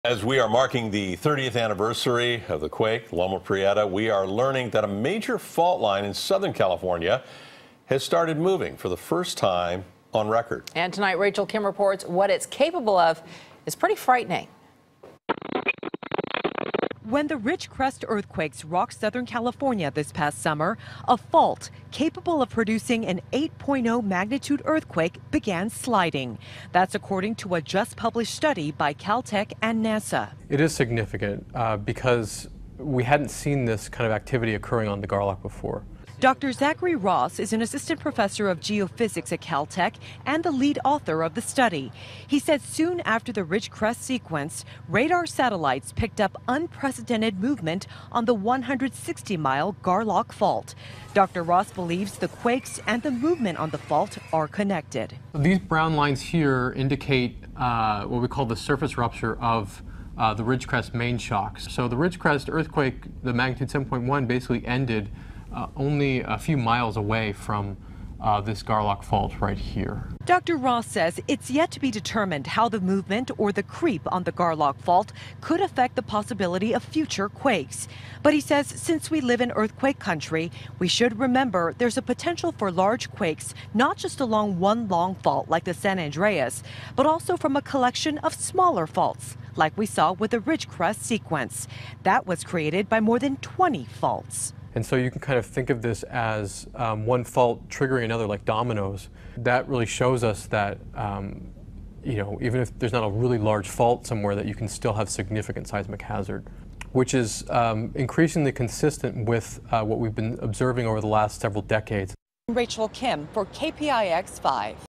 Dangerous SoCal Fault Begins Movement for First Time in Recorded History
The Garlock Fault, a major fault in Southern California, has started moving for the first time on record and, Rachel Kim reports, what it's capable of is pretty frightening. (10-17-19)